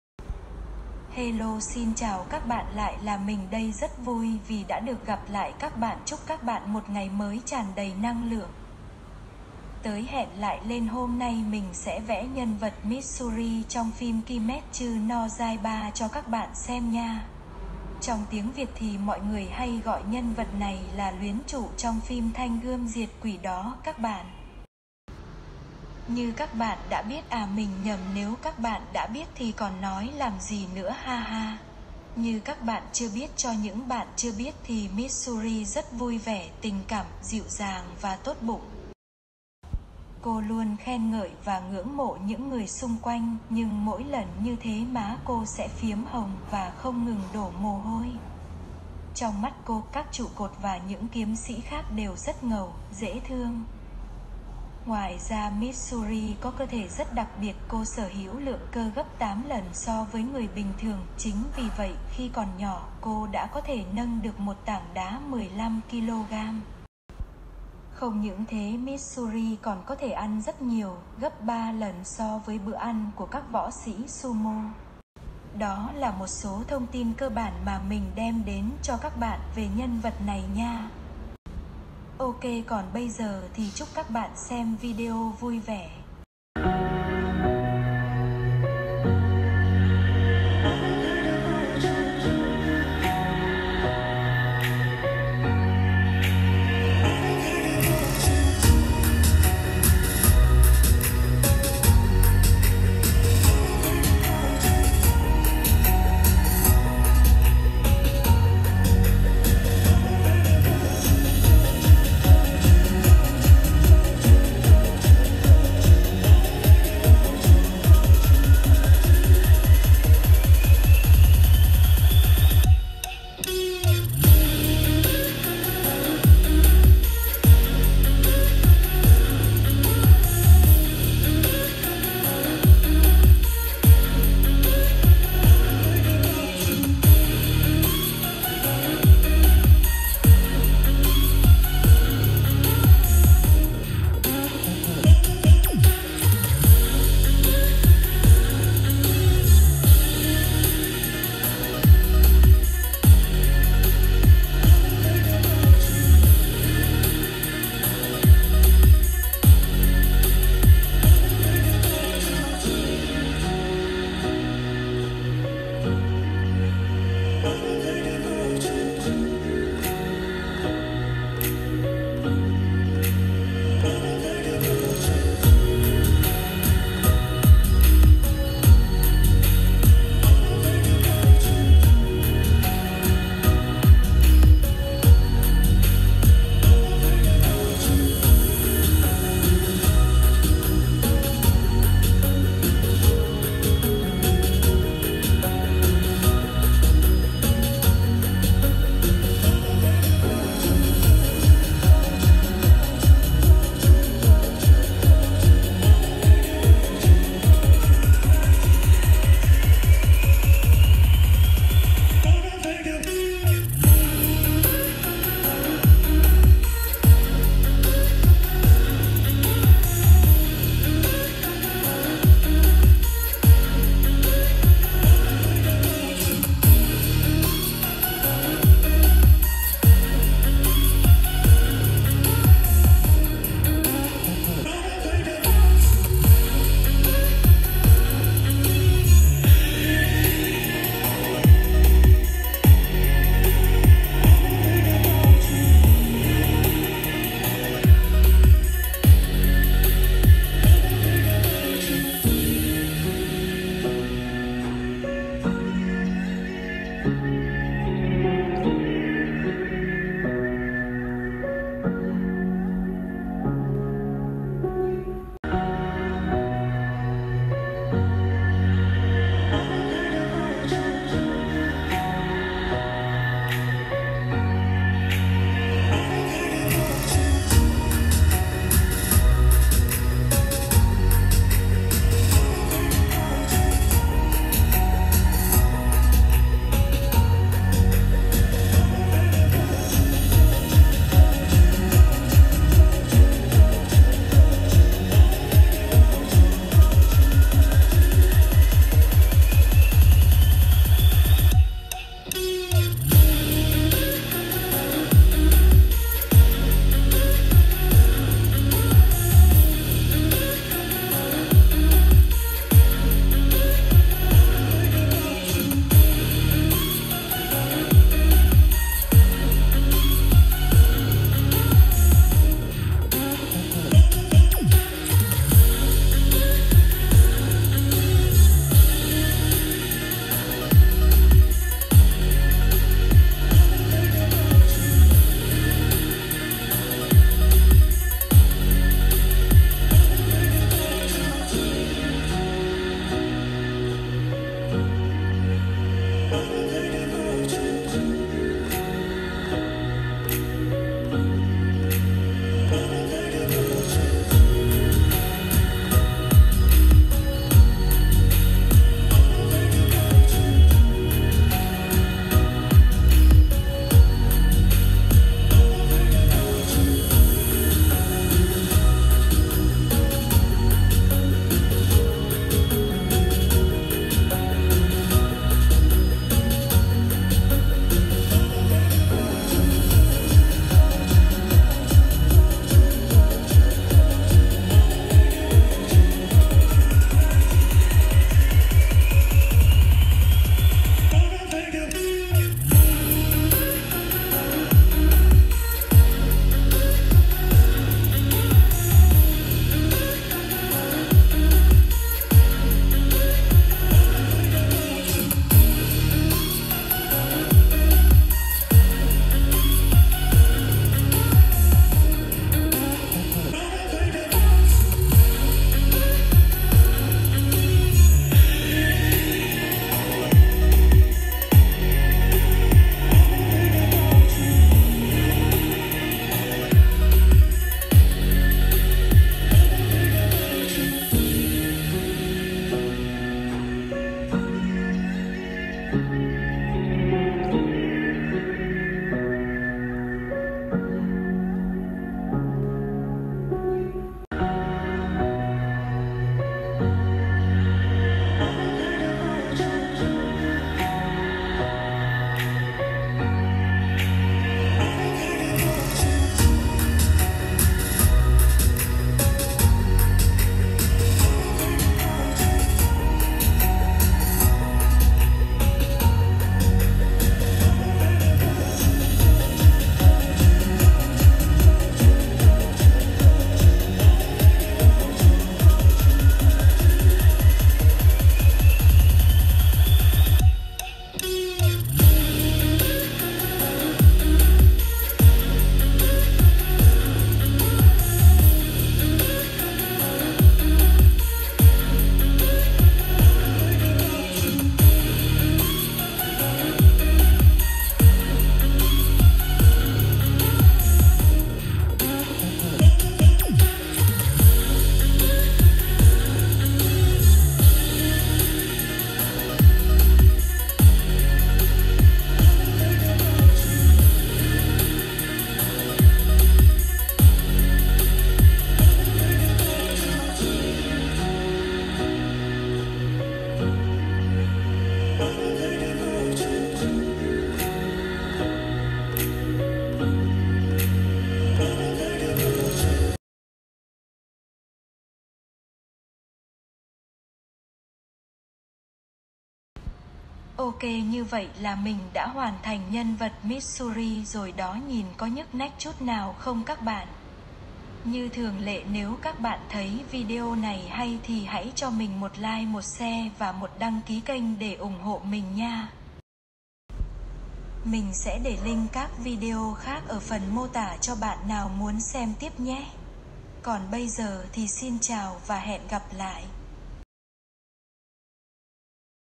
Vẽ Mitsuri (luyến trụ) trong Kimetsu No Yaiba
nhân vật hôm nay Mitsuri (luyến trụ) trong Kimetsu No Yaiba (thanh gươm diệt quỷ)
Mong rằng các bạn sẽ thích chiếc Video này. :)))